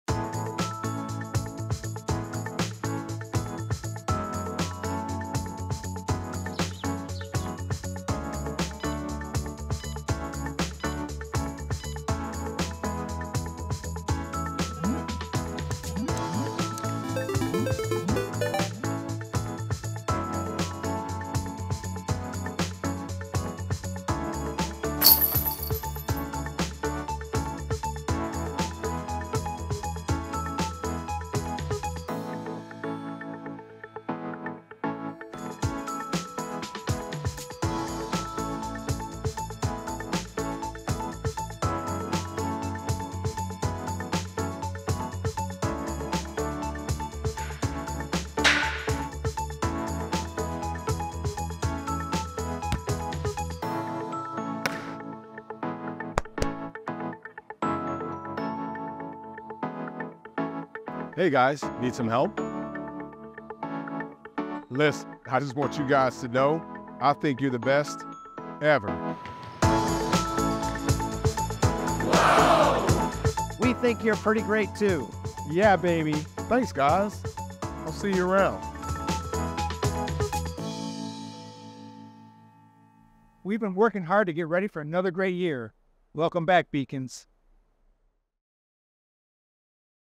Welcome Back 2023 | Valparaiso University
We are ready for another great year at Valparaiso University! Welcoming back our new and returning Beacons calls for a special message from a surprise guest. Join Beacon and Blaze (and whoever is in the mascot suits) for a trip around campus and a fun run-in with our very own Coach Roger Powell Jr. '16 M.S.!